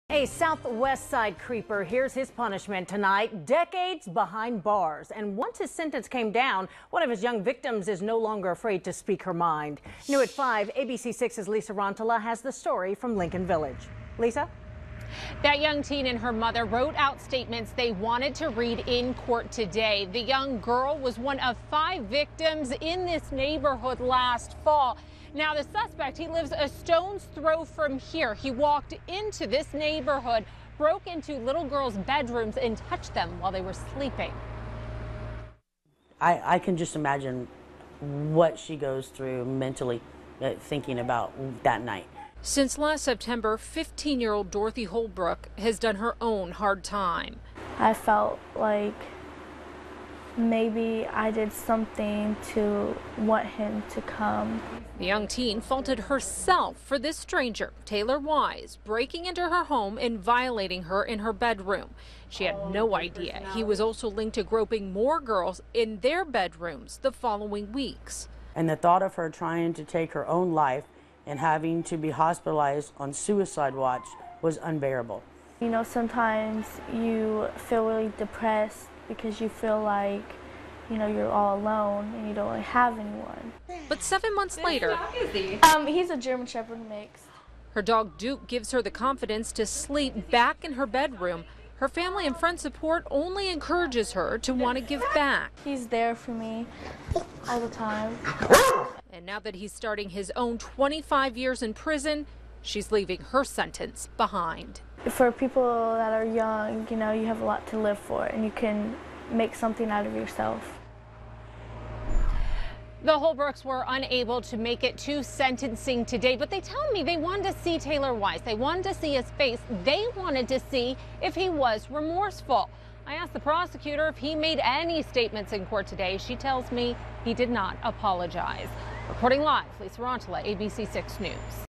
Lincoln Village Creeper Sentenced, Teen Victim Takes Her Life Back
LINCOLN VILLAGE (Lisa Rantala/Sean Rowe) -- The man convicted of breaking into a series of homes to fondle women and teenage girls last fall received a 25-year prison sentence in Franklin County court Monday.

Taylor Wise, 21, of Prairie Township, entered a plea deal in court to up to five different burglaries, which began last September.

Dorothy Holbrook, 14, was Wise's first victim. Speaking to ABC6/FOX28's Lisa Rantala, she says she who woke up one night last September to an intruder touching her body.

After the man got away, Holbrook says she could no longer sleep in her bed, started missing days at school and even tried to take her own life.

April Holbrook, Dorothy's mother, said, "I can just imagine what she goes through mentally, talking about that night."

Dorothy said, "I felt that because I didn't know him, I did something to want him to come."

Holbrook says by undergoing extensive counseling and with the strong support of her family, she was able to write a statement to read during Wise's sentencing hearing. She let him know that he did not break her spirit and that and he no longer had control over her.

Dorothy's also made a new friend since the attack.

"He's a German Shepard mix," she said. "He's there for me all the time."

Dorothy says her friend, Duke, gives her the confidence to sleep in her bedroom again. Support from her family and friends has encouraged the teen to want to give back. She's involved in a school program that helps other troubled teens who may have been victimized in some way.

"For people who are young, you have a lot to live for and you can make something of yourself."